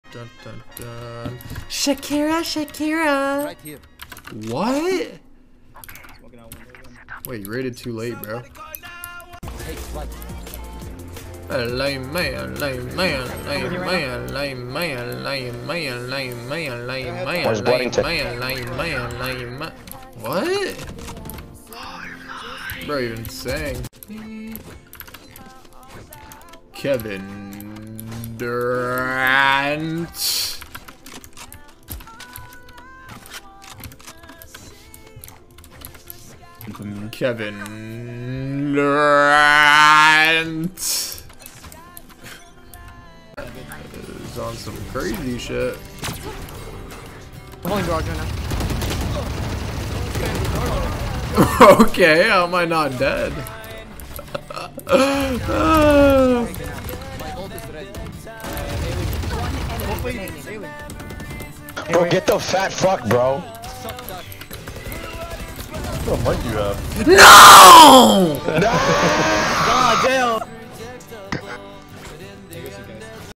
Sinatraa experiences "Ranked Brain" and has fully lost his mind...
Bro is just saying some random shit while attempting to aim, you gotta love it 🤣